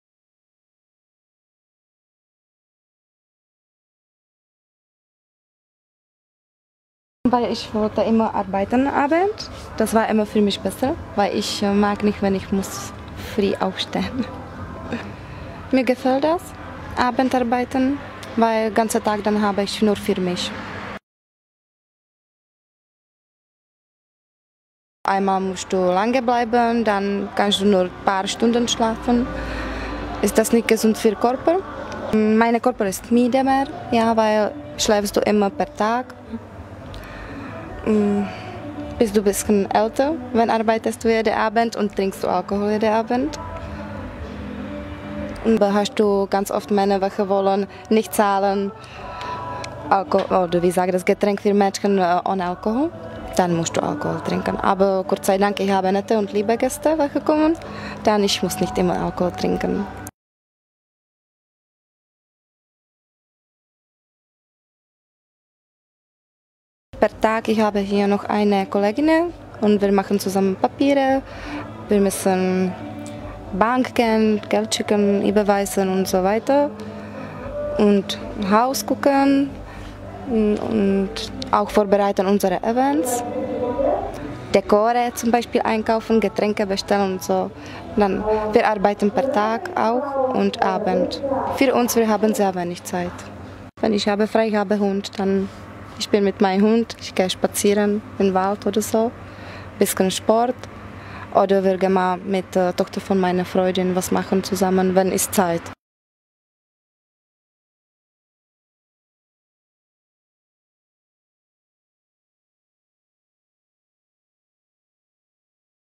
Nacht in Franken - Zu Besuch im Stripclub
Nacht in Franken: Arbeiten im Stripclub

Kamera und Schnitt: Robert Huger